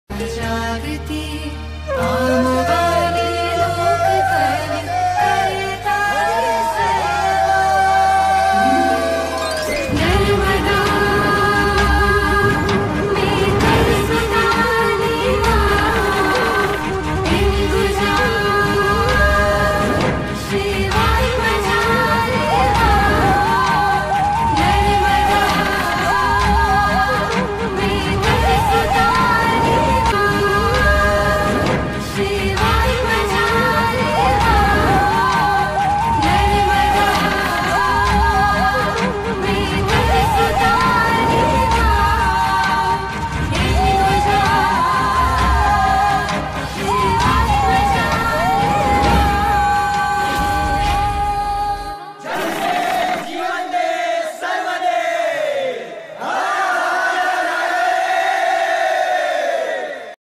*ये महैश्वर का विडिओ है भाईयो जिसमे मॉ नर्मदा जी को चुनरी चडा़या गया है,विडियो देखे*
🙏 *जय हो नर्मदा मैय्या की*🙏